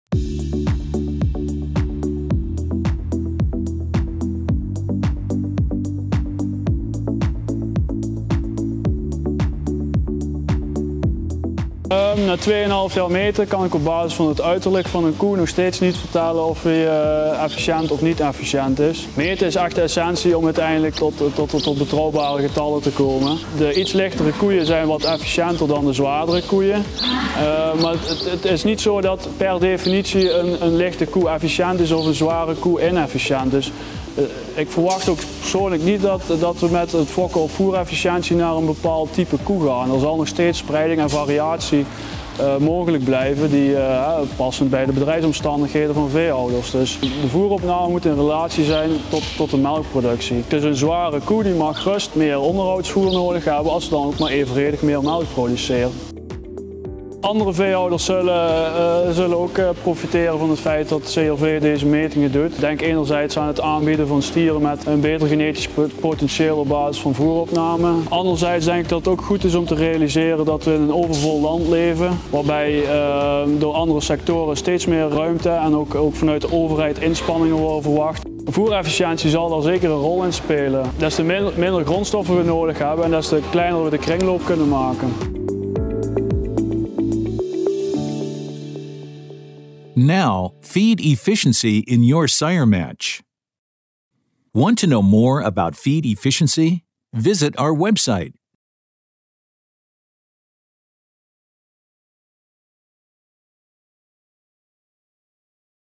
Testimonio agricultor Alders
En agosto de 2017, la granja lechera dirigida por la familia Alders se convirtió en el primer sitio de pruebas de CRV en medir el consumo de pienso de animales individuales. “Damos la bienvenida a nuevos desarrollos y esta granja tiene una larga relación con CRV”, explica Willem Alders (29), quien tiene un rebaño de 200 vacas lecheras y vacas embarazadas y 90 cabezas de ganado joven en Overloon, Países Bajos. En un inicio fue escéptico. “El concepto de eficiencia alimentaria no es algo que realmente capture la imaginación aun en la producción de lácteos. El tipo de granja y la estructura de edades del rebaño también tienen una significativa influencia en la eficiencia alimentaria”, comenta Alders. Sin embargo, sintió entusiasmo por el valor de crianza durante una visita al Dairy Campus en Leeuwarden. “La diferencia entre los animales individuales resultó ser enorme. Y tampoco se puede ver a simple vista. Medir realmente es saber: esa es la esencia de la historia”.

SPA LA